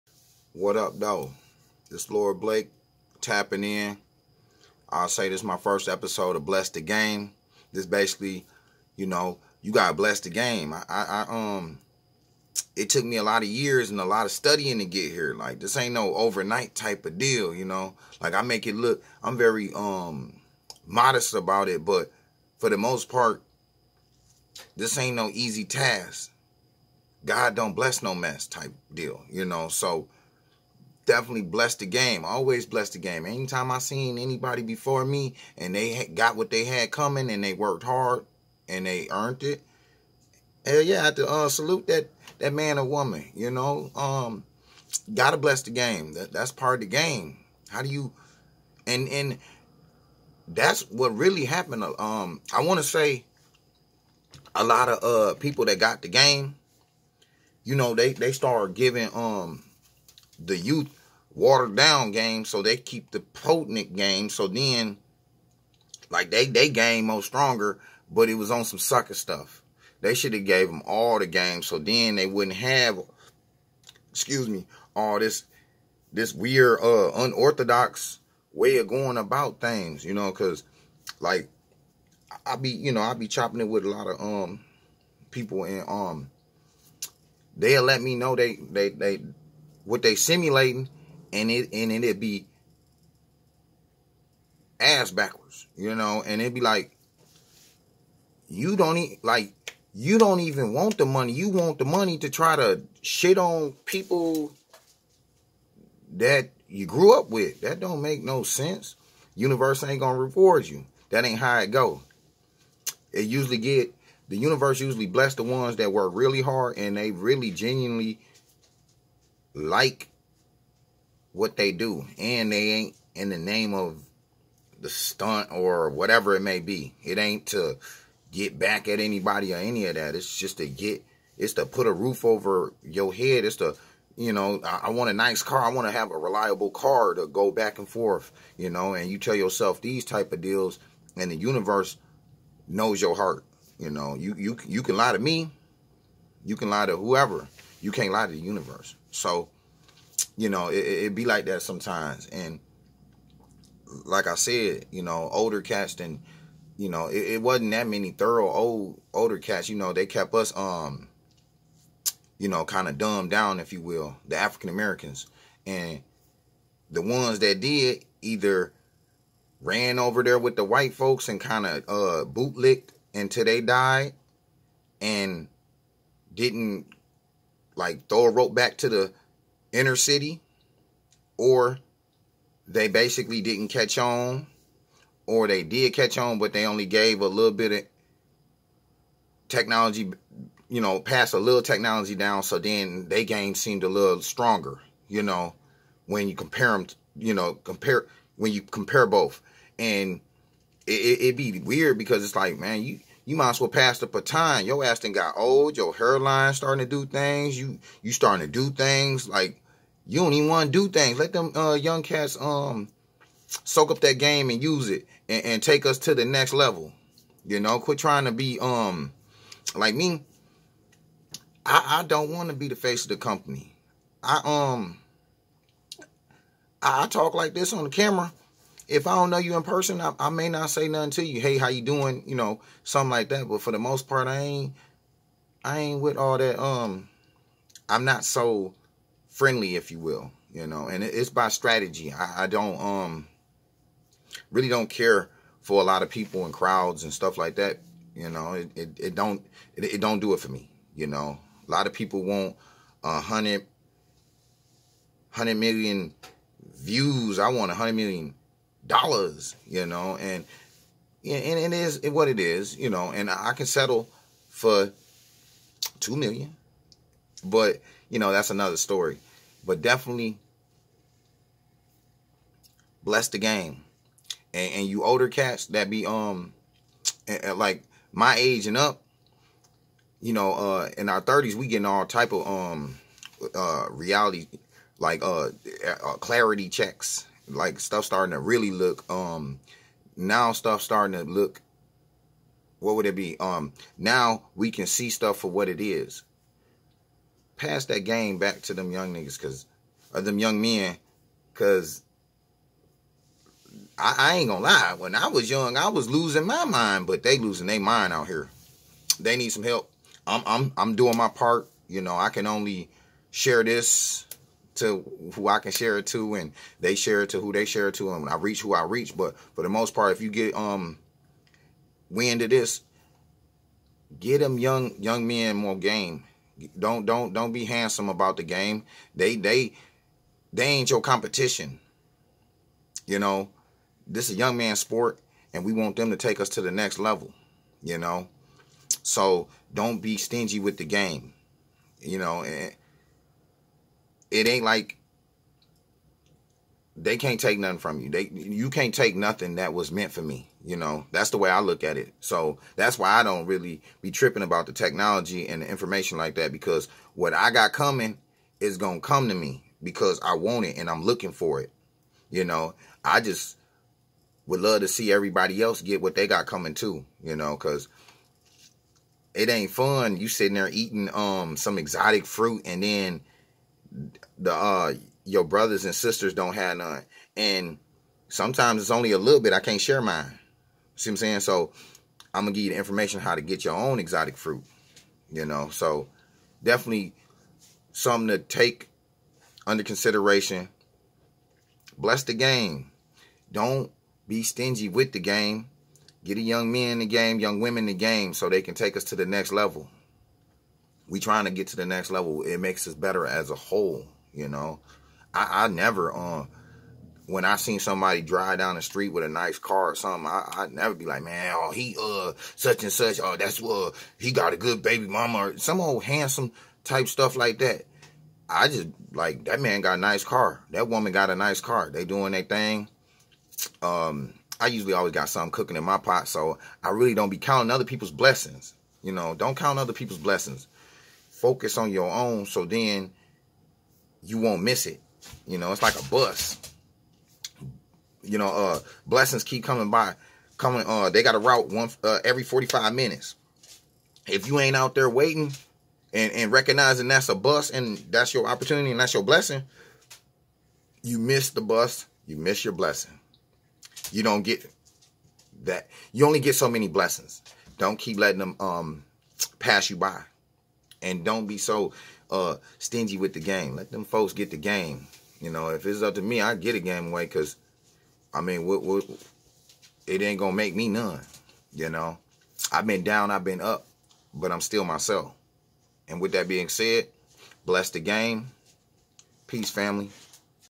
Bless The Game Episode 1 Don't Be Stingy With The Game
We need to stop being so stingy with the game aka the knowledge.